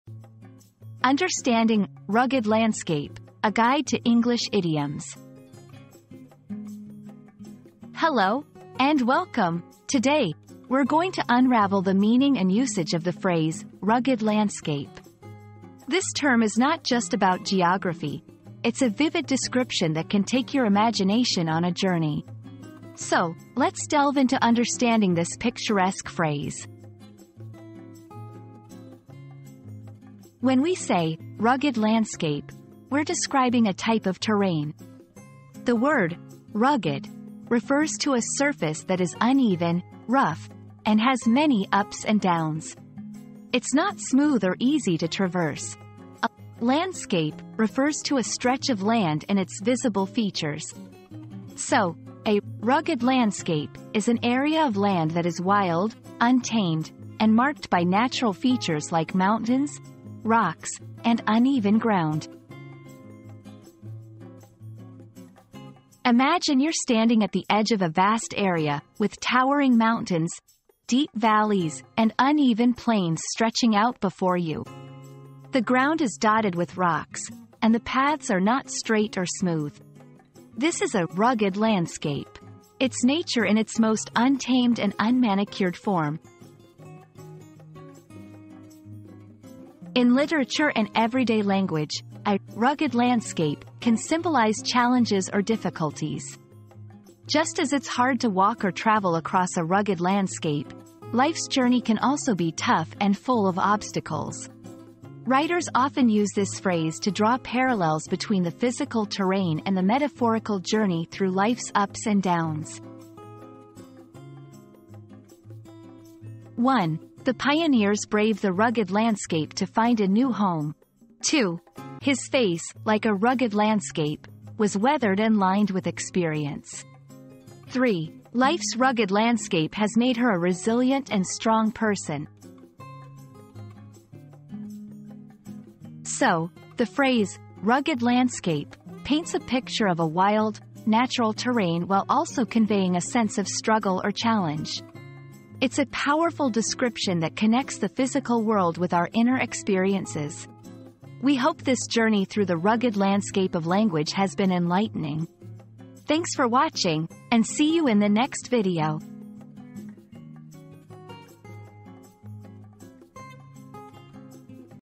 Understanding "Rugged Landscape": A Guide to English Idioms
00:00 • Introduction - Understanding "Rugged Landscape": A Guide to English Idioms
00:29 • The Literal Meaning of "Rugged Landscape"
01:05 • Visualizing a Rugged Landscape
01:29 • Symbolic Meanings and Usage in Language
01:56 • Examples in Sentences